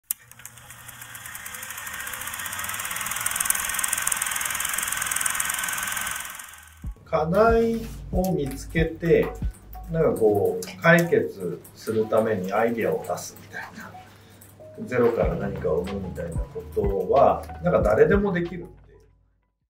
誰もが社会を変えられる | 株式会社プラゴ 代表取締役大川直樹 | FOCUS ON FILM ー 起業家・経営者の人生に迫る
小さく漸進的な変化が積み重なれば、やがて大きなイノベーションとなる。

「続けたくなる未来」を創造すべく、EV（電気自動車）を取り巻くあらゆる領域のサービスをアップデートしていく株式会社プラゴ。同社が取り組む領域は、充電中や前後も含めた体験価値のデザインを中心に据えた、オンライン予約・決済が可能なEV充電サービスや、法人向けのEV充電ビジネスに関するソリューション提供など幅広い。2023年には、ニトリグループ、ヤマダデンキ、ミニストップとの提携を開始するなど、社会インフラとしてパートナーシップを拡大している。

代表取締役の大川直樹は、慶應義塾大学法学部を卒業後、株式会社電通へ入社。マーケティング業務や、子会社であるインタラクティブ・プログラム・ガイド（現 IPG）社にて社長直下での事業開発などに従事。のち家業である大川精螺工業株式会社ではメキシコ現地法人・工場立ち上げ、代表引き継ぎなどを経て、2018年に株式会社プラゴを設立した。同氏が語る「黒子の心」とは。